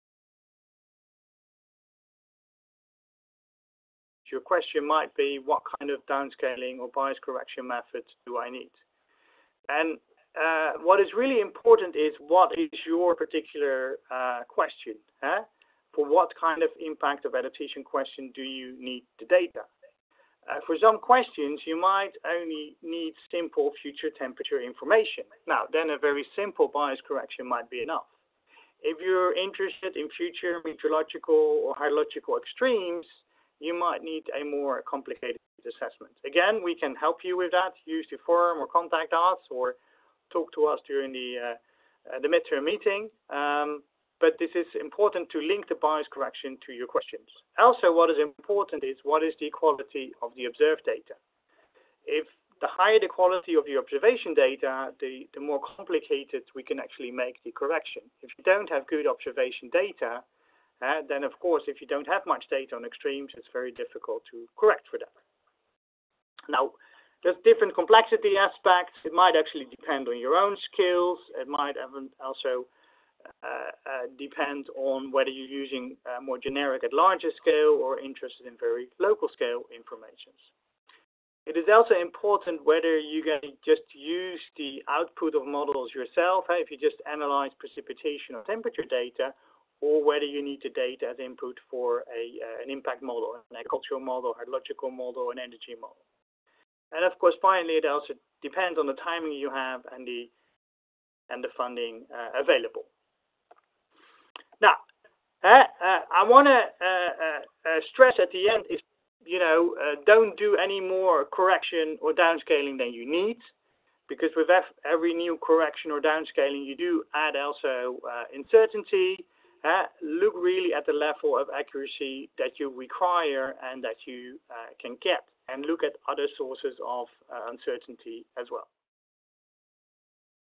How to select a bias adjustment method
Professor Fulco Ludwig (Wageningen University) highlights the most important issues to consider when selecting what adjustment/correction methods to use when working with climate data.